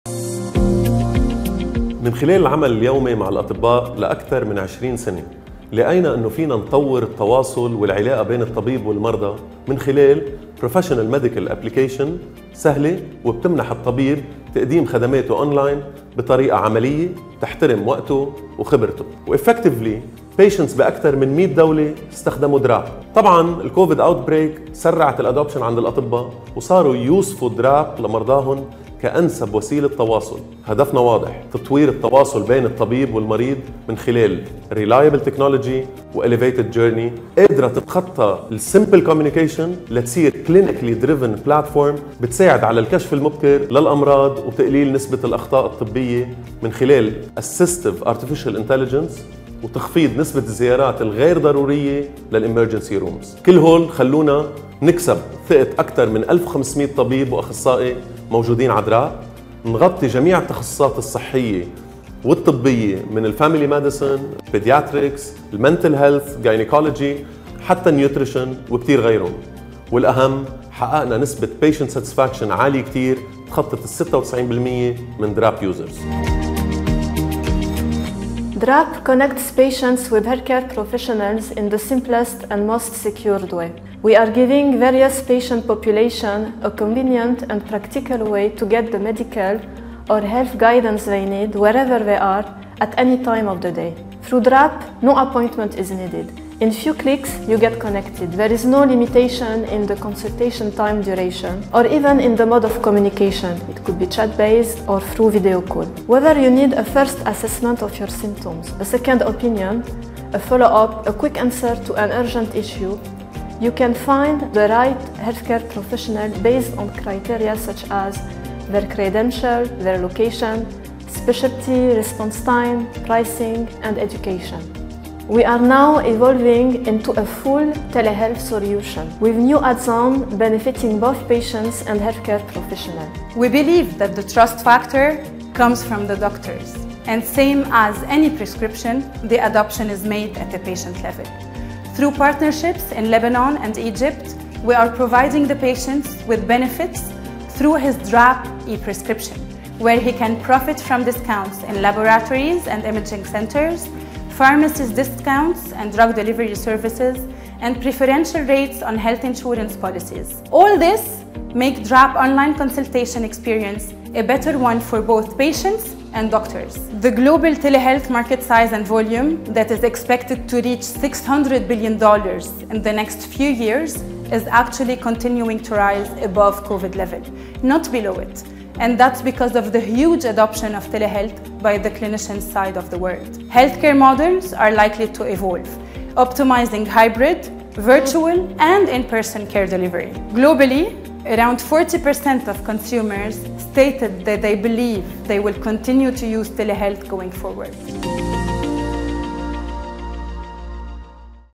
DRAPP, Telehealth made easy
DRAPP Telehealth journey described from business, product, and benefits perspective.
Learn more about how it all started, the evolution of telemedicine, and its positive impact on the healthcare ecosystem.